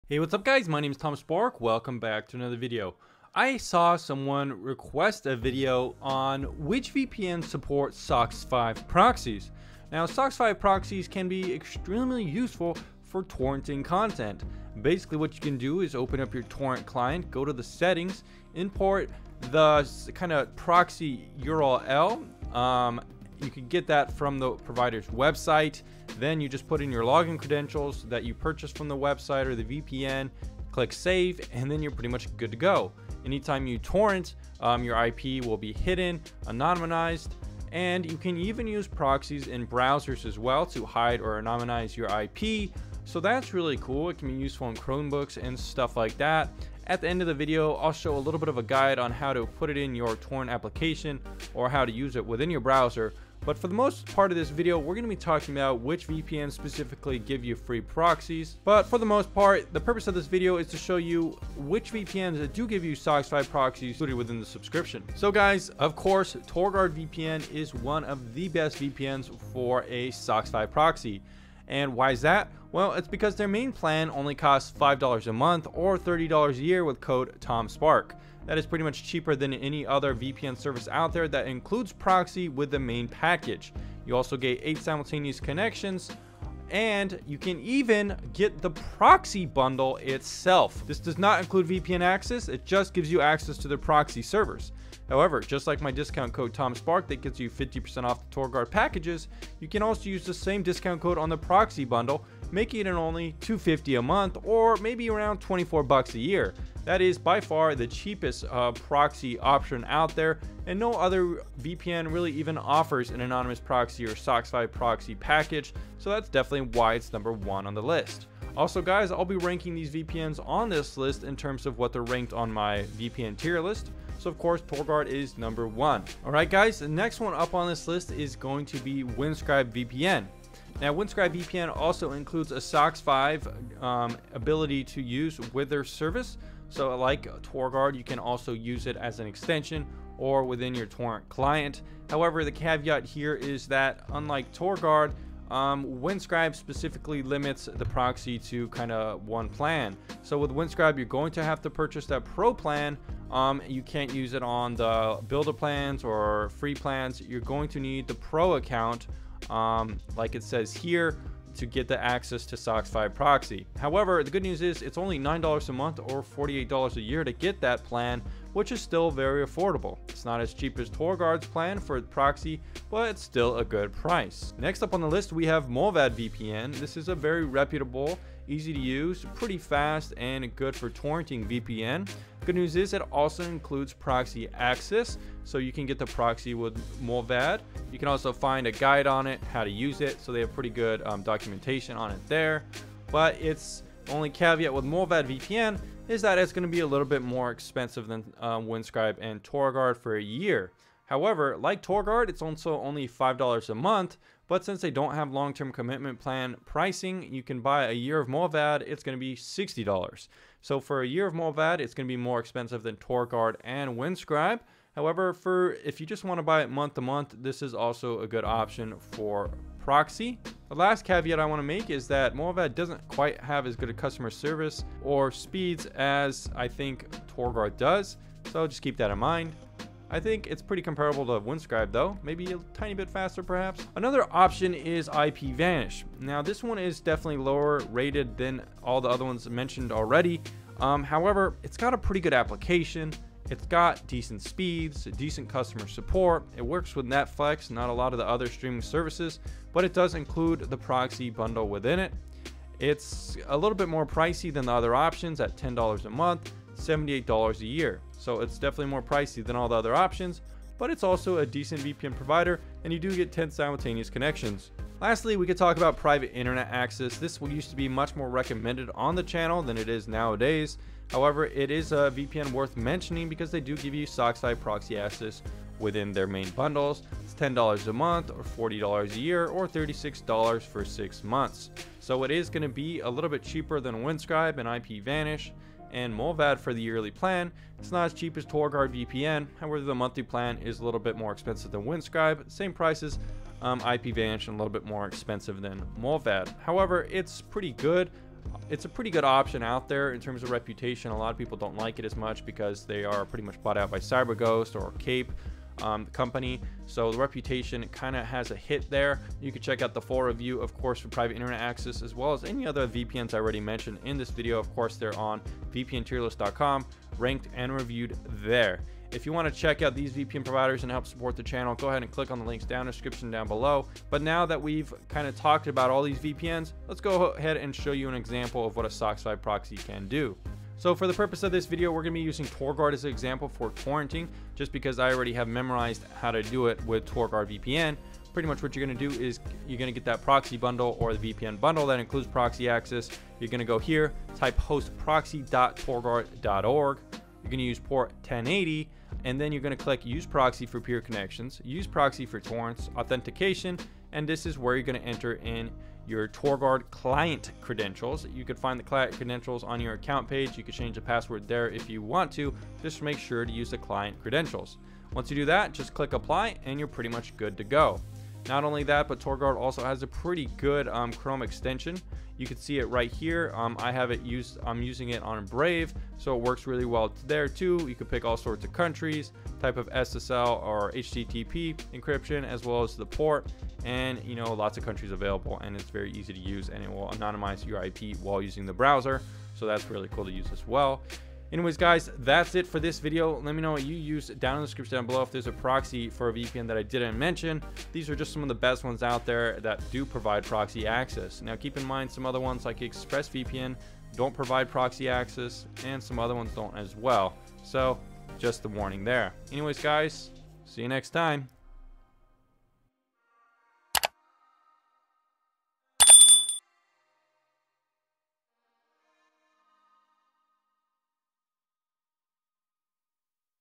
Top 5 VPNs that have Socks5 Proxies!
Which VPNs have the best SOCKS5 proxies? Find out now!

►Have you seen my favorite products page? Check it out to see my most recommended products!